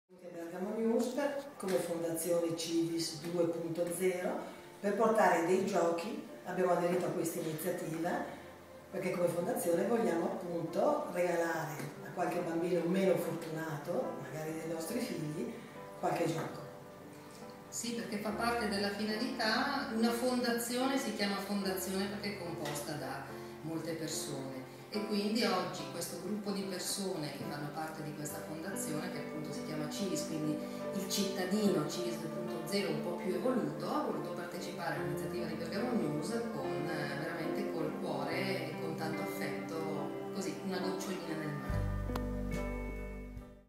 La solidarietà di Civis 2.0 verso la raccolta giochi di BgNews
Civis 2.0, la neonata fondazione guidata da Alessandra Gallone, si è schierata a fianco di BgNews per la raccolta giocattoli destinata ai bambini meno fortunati: "Proviamo a far ripartire il territorio anche con questi piccoli ma importanti gesti".